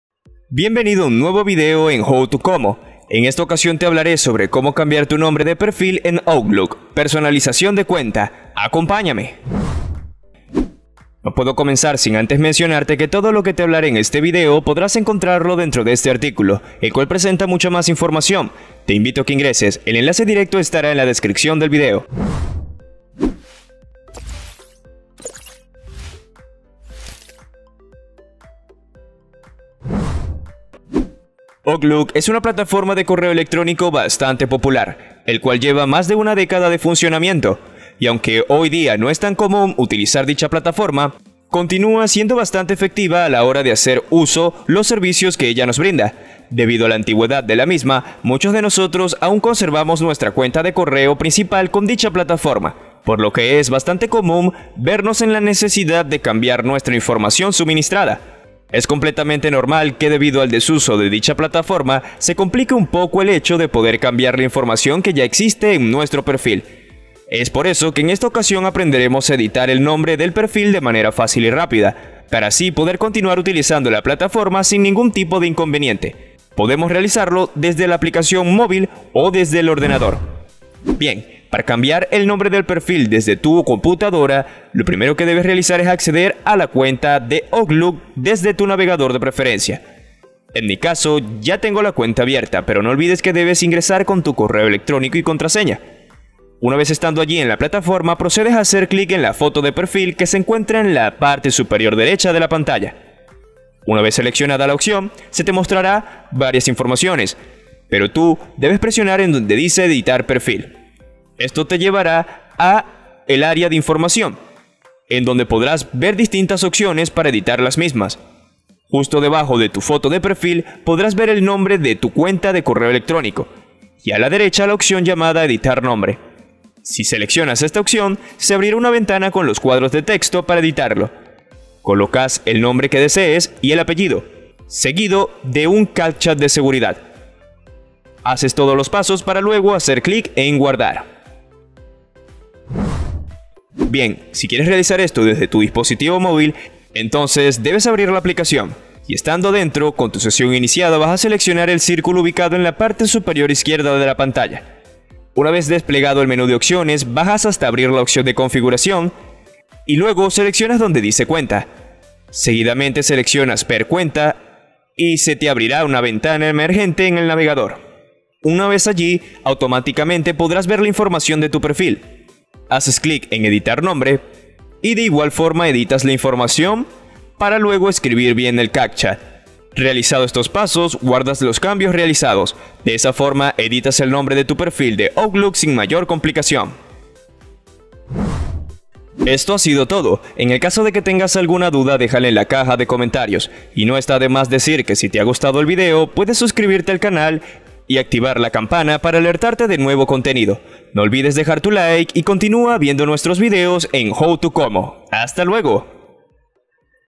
¿Cómo cambiar mi nombre de perfil en OUTLOOK? - Personalización de cuenta
En este video te mostramos ¿Cómo cambiar tu nombre de perfil en OUTLOOK? - Personalización de cuenta. En este canal encontrarás los mejores tutoriales. ¡No te lo pierdas!